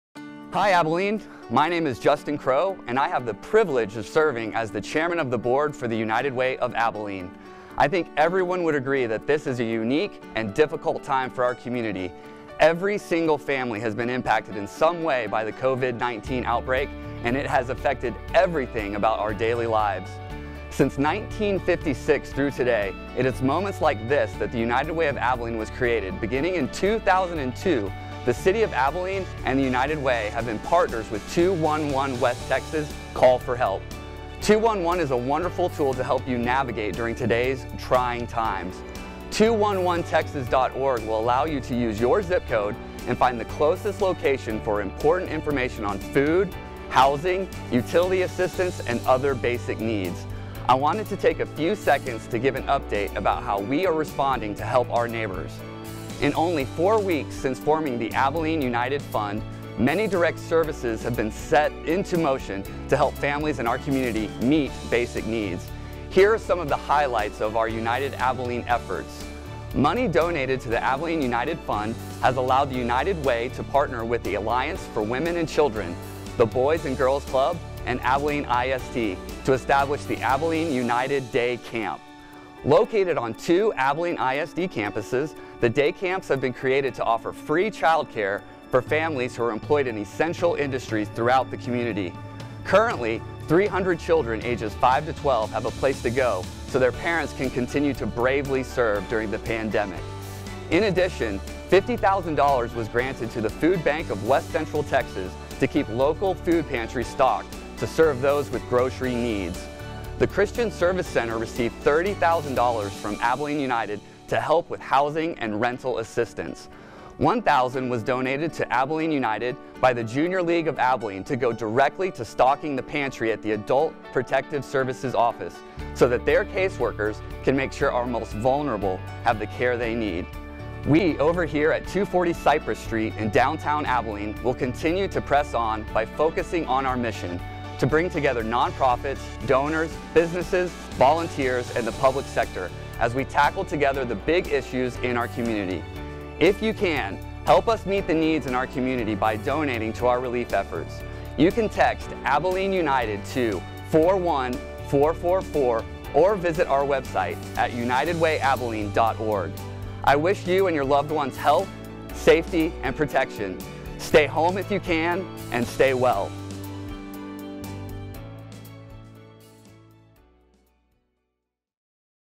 How the United Way of Abilene is helping the community during coronavirus pandemic
Hear from board chair Justin Crowe